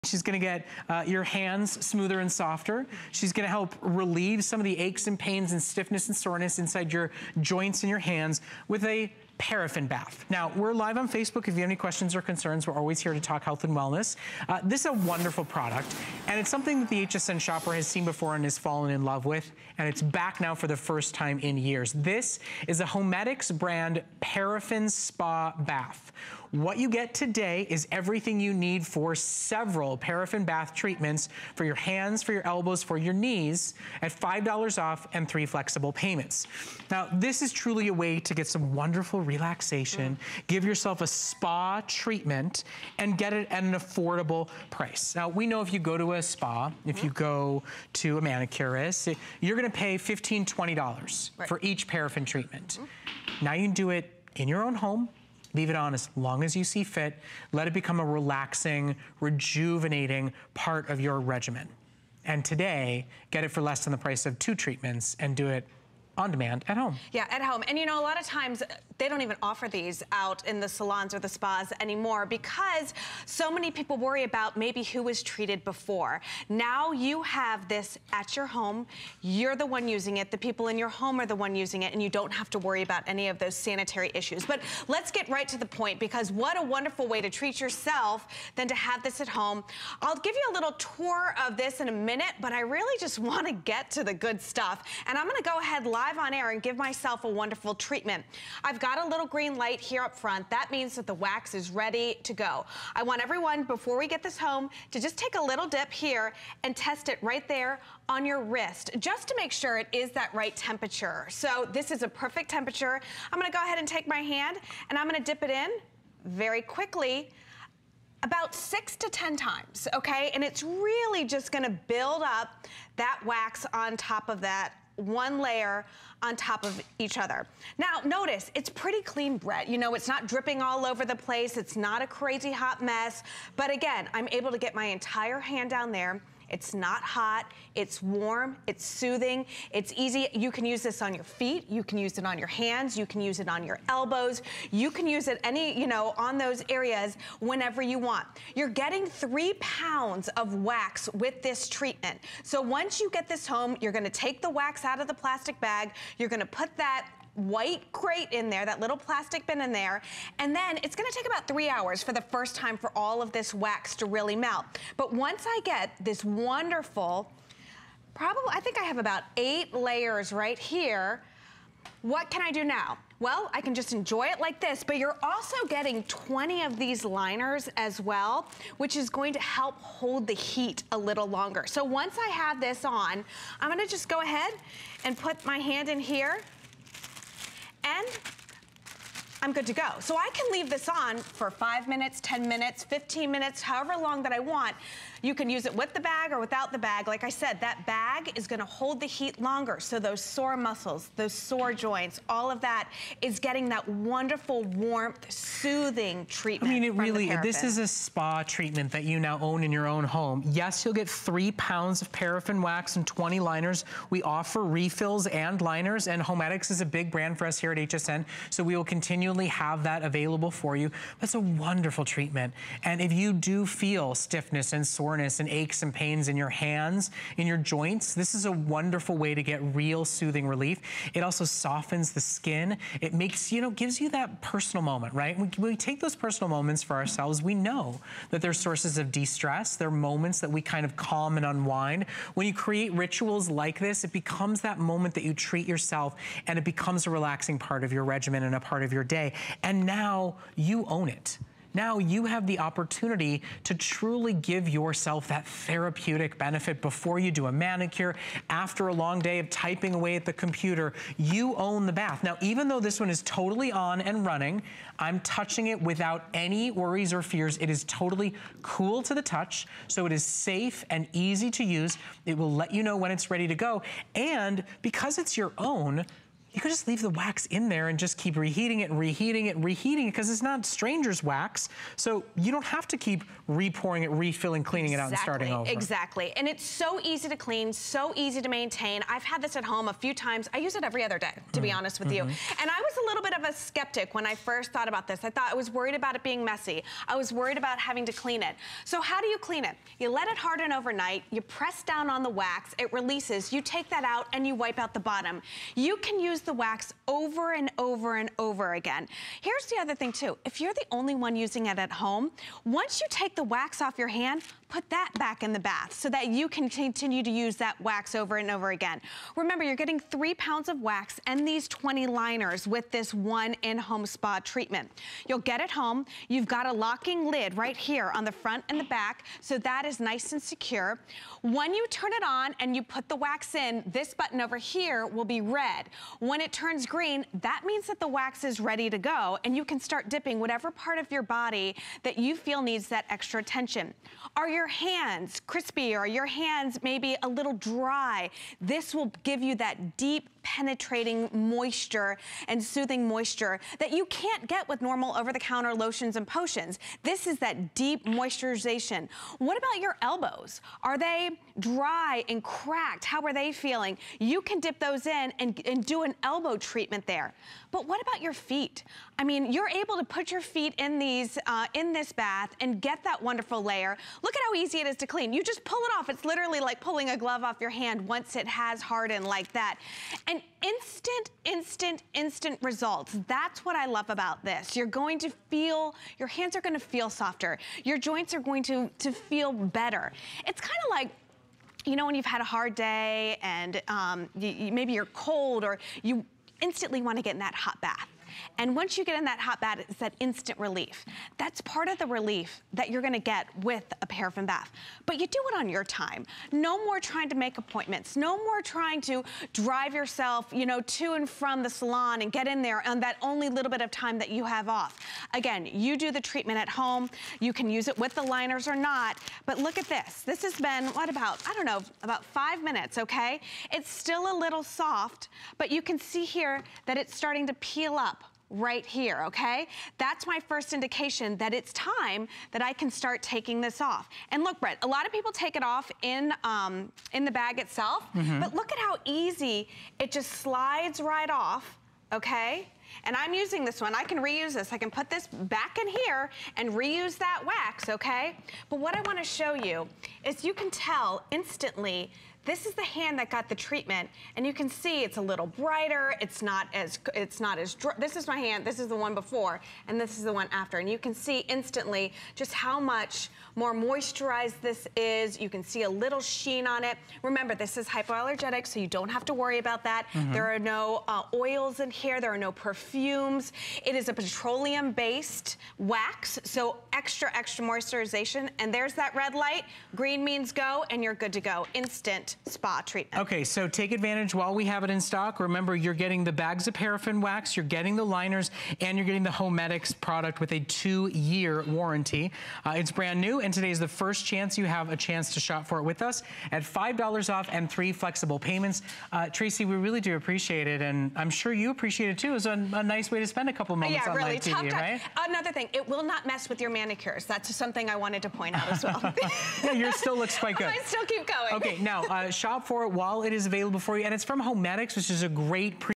HoMedics ParaSpa Plus Paraffin Bath
Soften dry skin while soothing tired muscles with paraffin wax and gentle heat. The cooltouch tub keeps the paraffin bath cooler to the touch than metal tubs,...